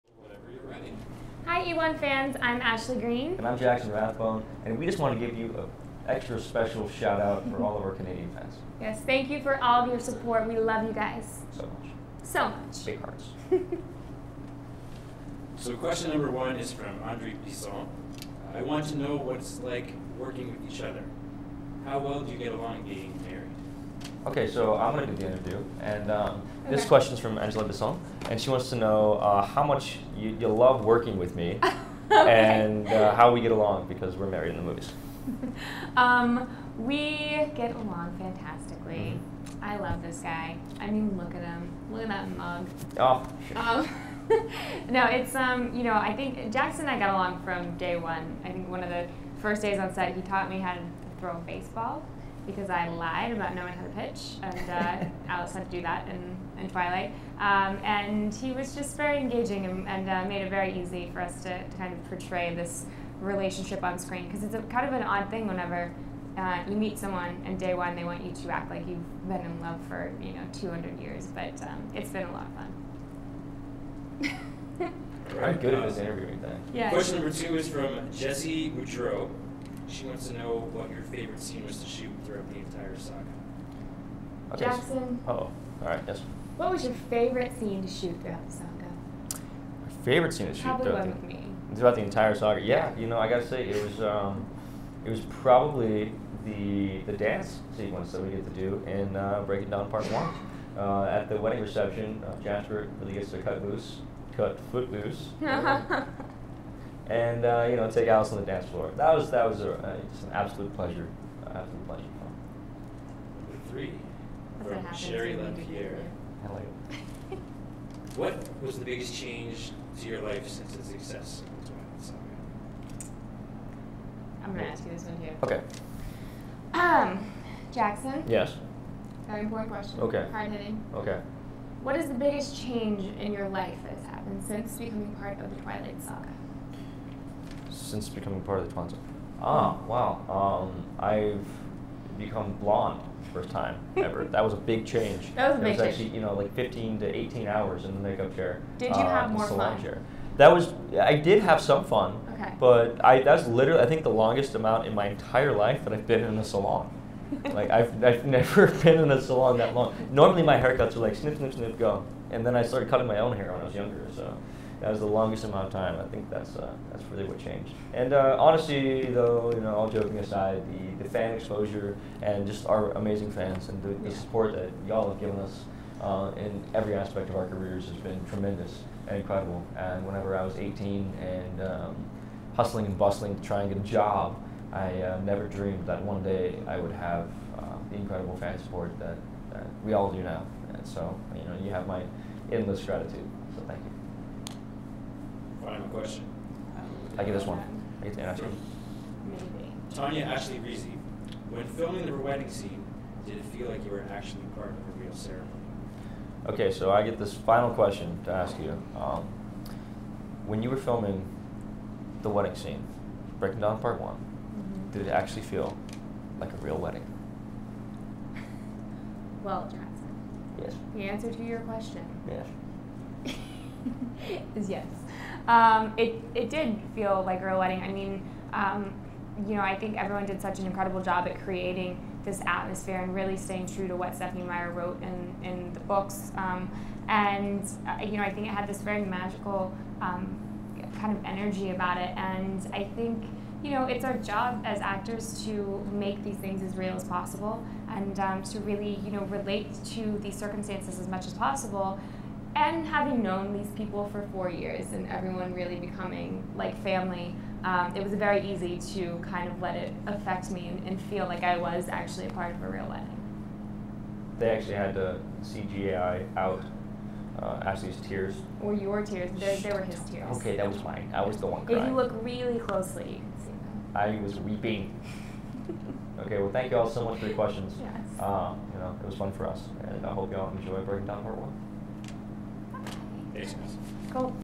The Twilight Saga: Breaking Dawn - Part 1 - Ashley Greene and Jackson Rathbone
Ashley Green and Jackson Rathbone answer fan questions at the Canadian premiere of The Twilight Saga: Breaking Dawn - Part 1